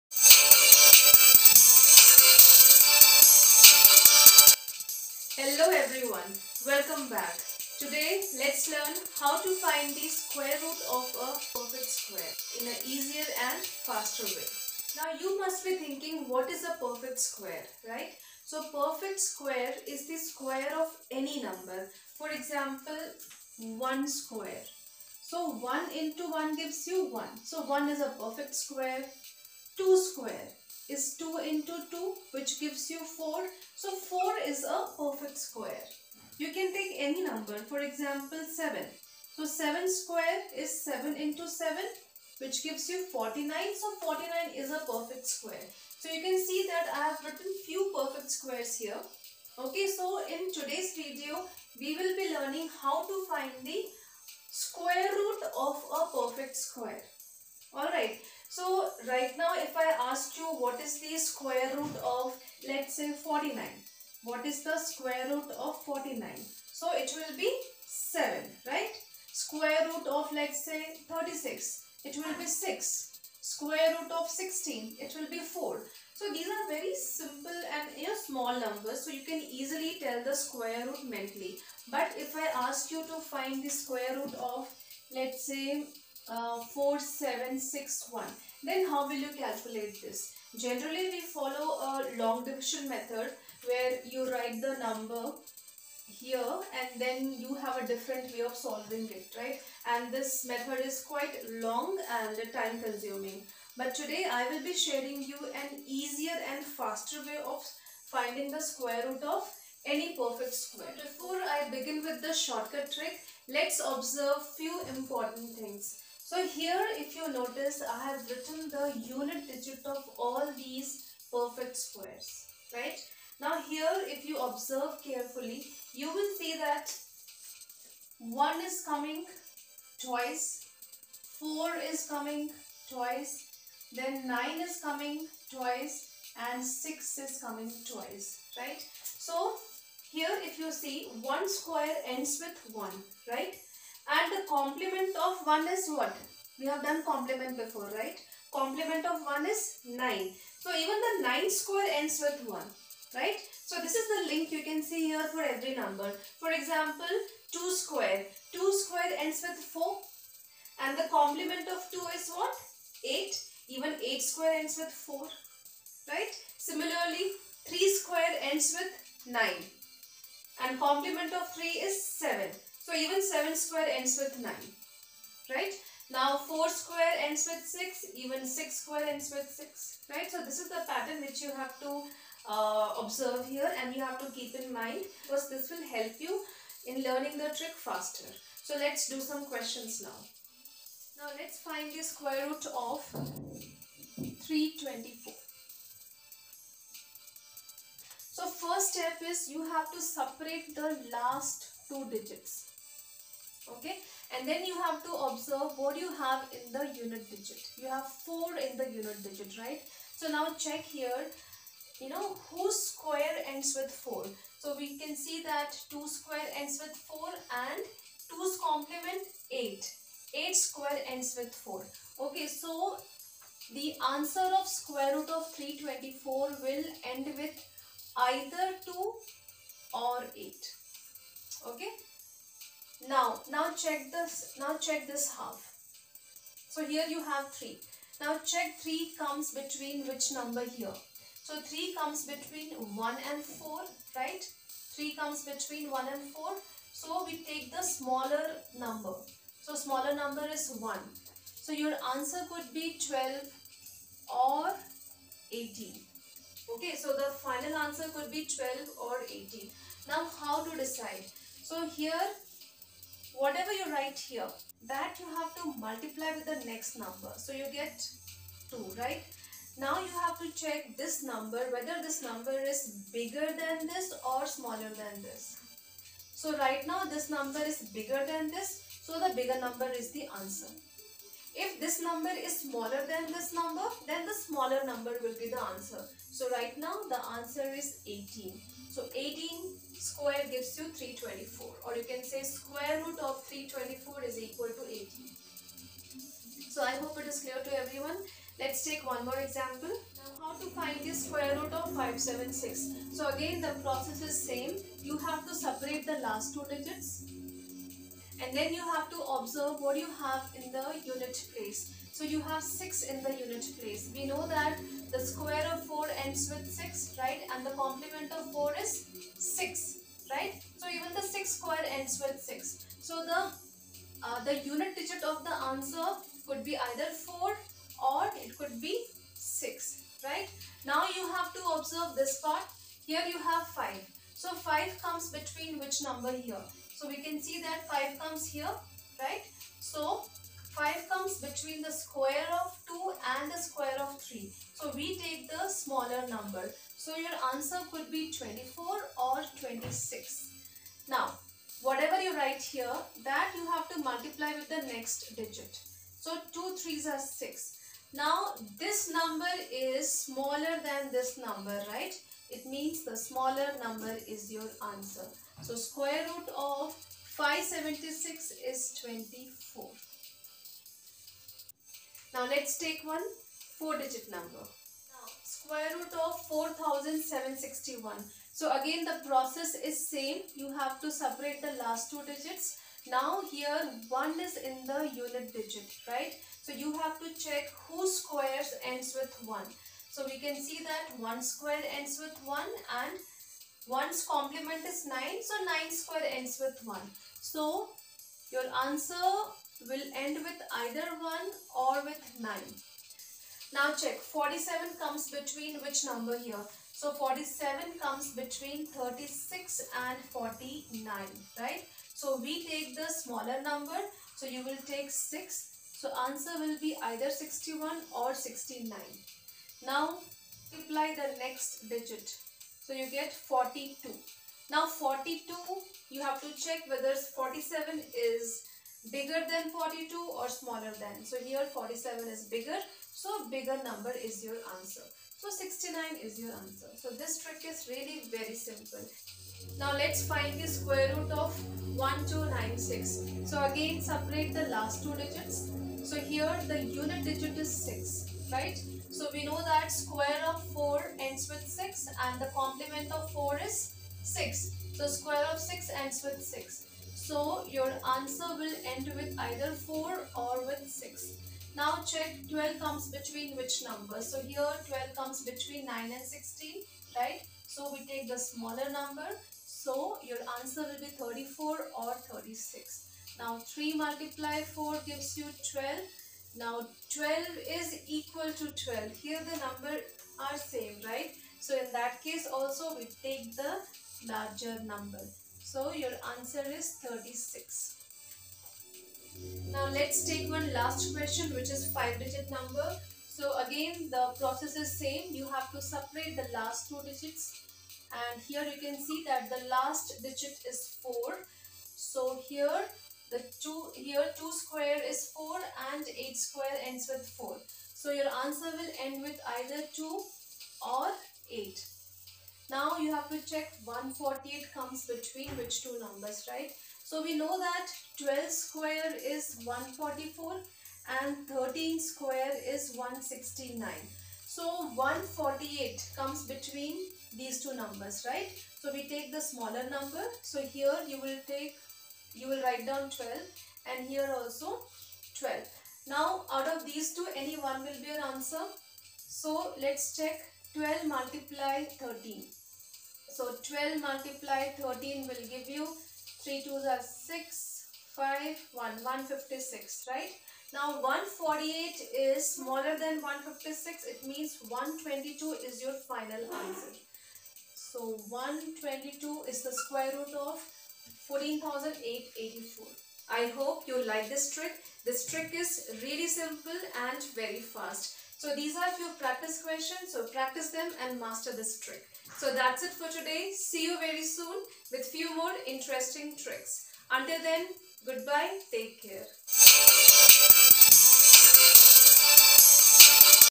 How to find the square root of a perfect square/Square root trick/Vedic maths/In seconds
Thanks for taking out time and watching my videos